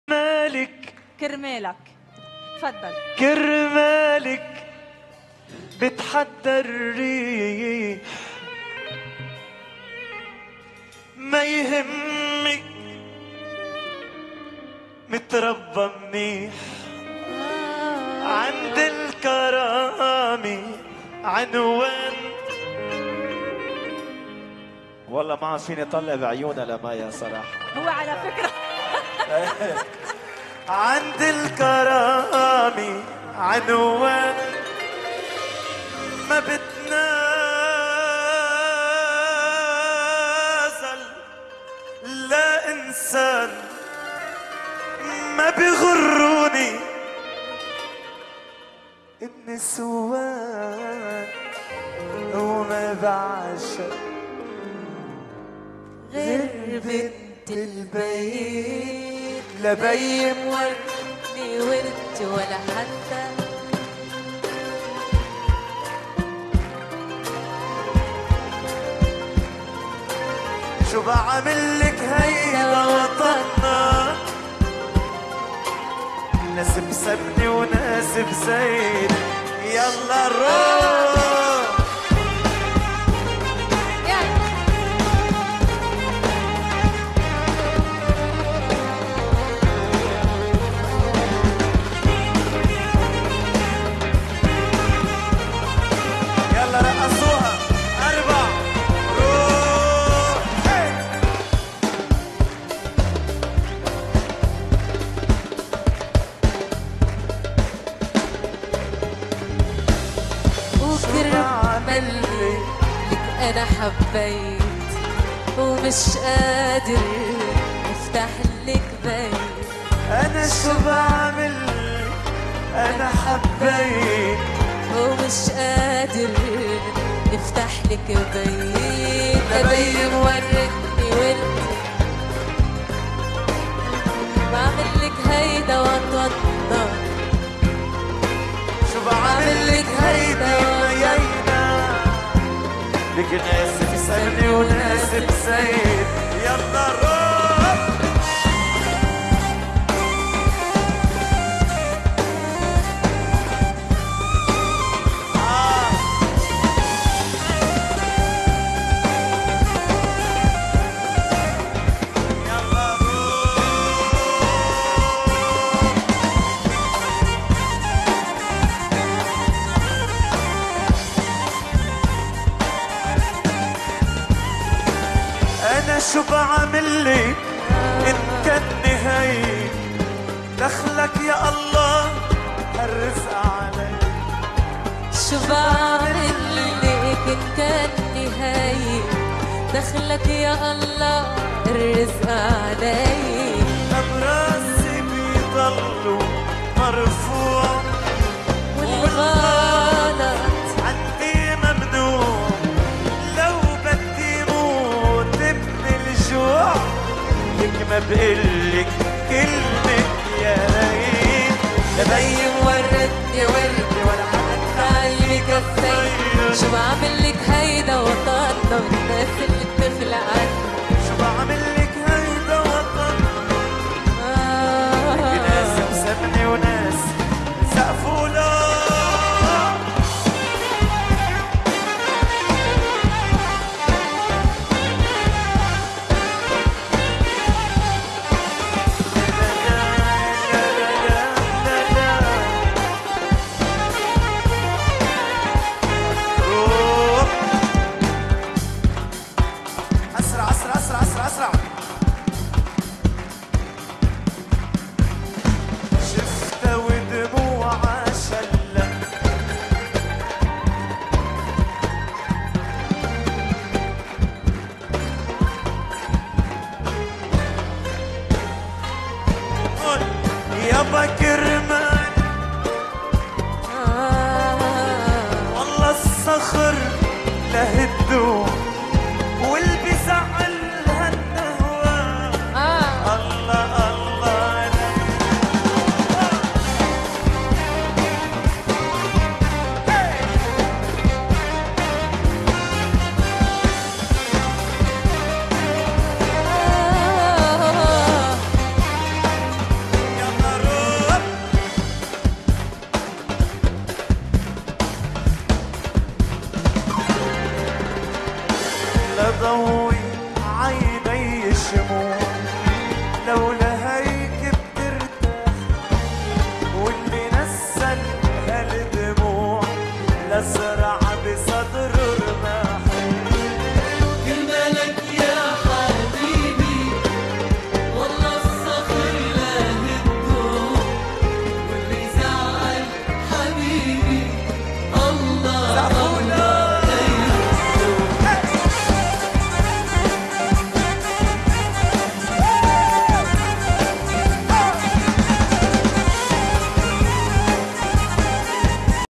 ايمن زبيب لمايا دياب: ما عم بقدر اتطلع بعيونك...
Entertainment program hosted by Maya Diab in which contestants compete in singing games.

برنامج ترفيهي مع مايا دياب ويتسابق فيه نجومٌ وهواة في ألعابٍ وأغانٍ